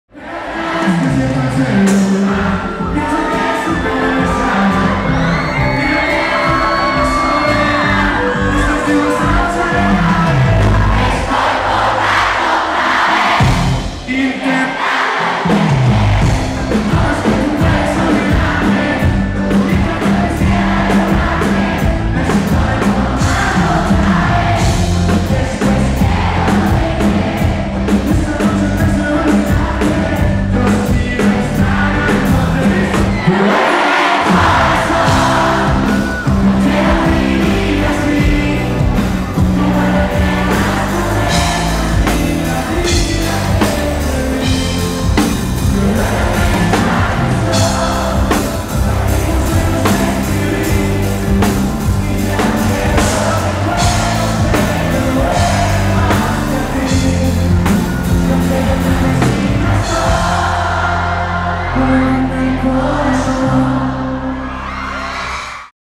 Sebastián Yatra en Querétaro | 14 de septiembre
Así fue el cierre de la gira de Sebastián Yatra por México, cerrando en Querétaro.